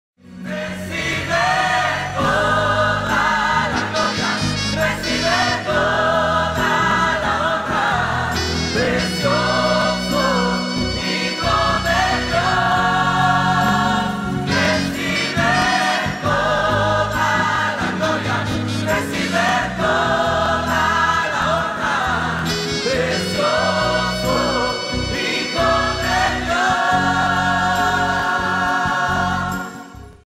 Semana Aniversario 25 Años - Marzo 2018
Desde el lunes 12 al Domingo 18 de marzo de este año 2018 celebraremos los 25 años de Pastorado en la IMPCH Maipú.